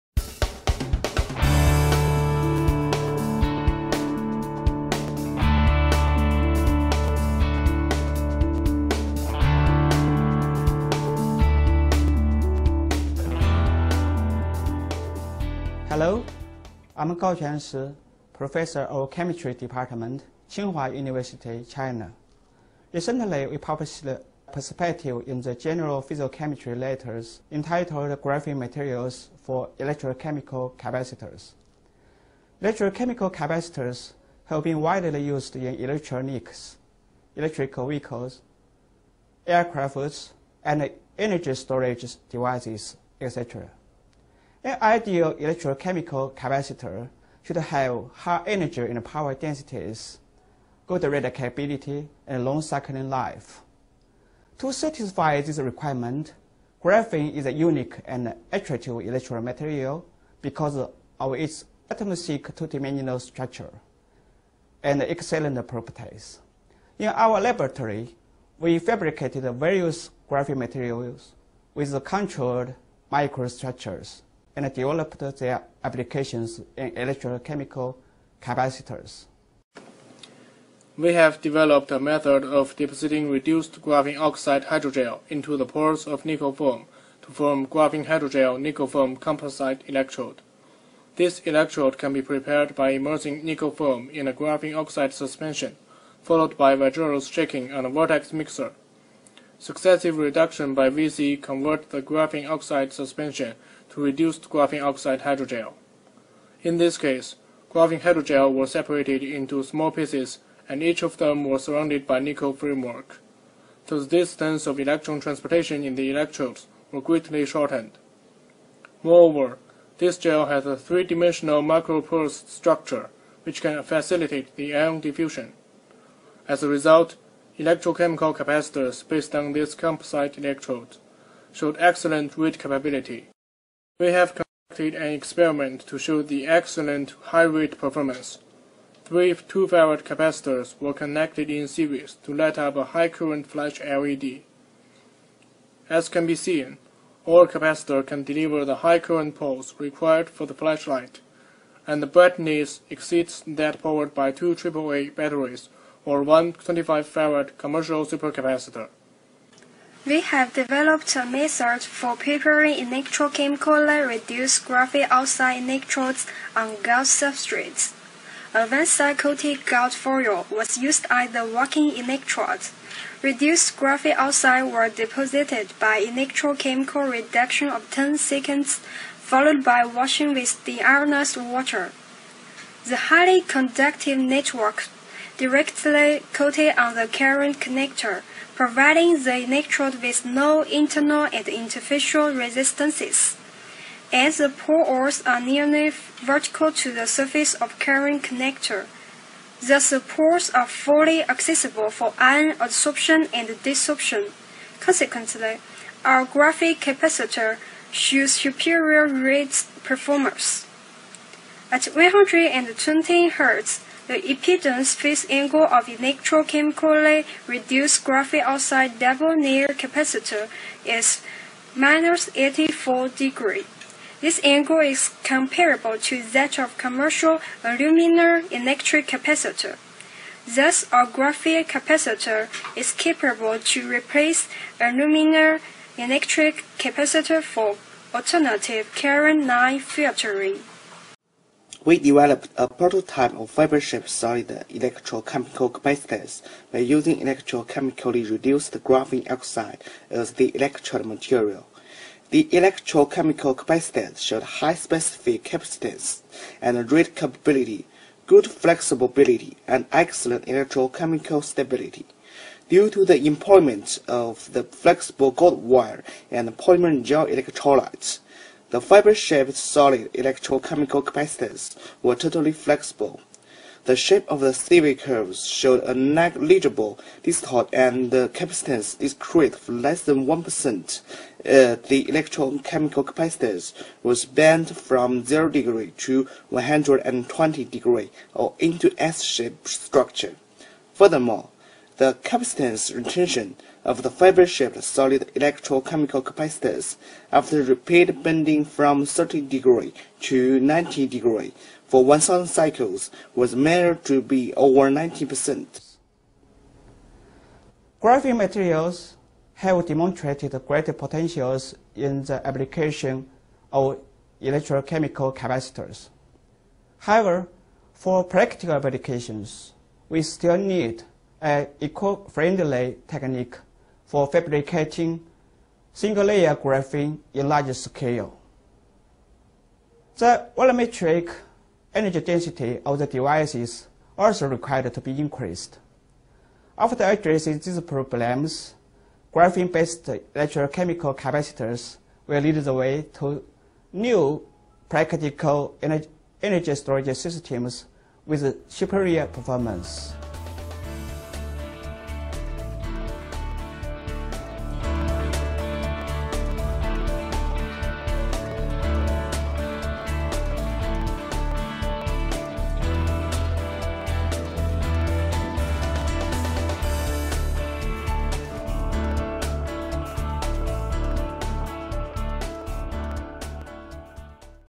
Graphene Materials for Electrochemical Capacitors
In this video, Ji Chen, Chun Li, and Gaoquan Shi, Department of Chemistry, Tsinghua University, Beijing, People's Republic of China discuss their Perspective published in issue 8 of the Journal of Physical Chemistry Letters.